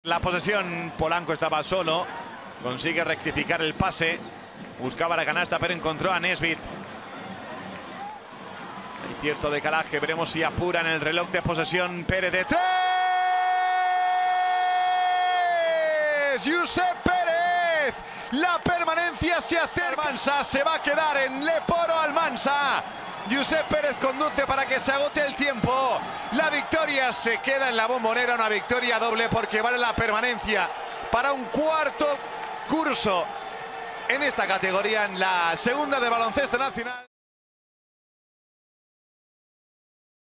LEB Oro | La permanencia del CB Almansa, a lo grande de la mano de Josep Pérez
Un triple de JosepPérez al borde de la conclusión del partido le dio al CBAlmansa la victoria en casa y la permanencia en la LEBOro haciendo estallar la Bombonera.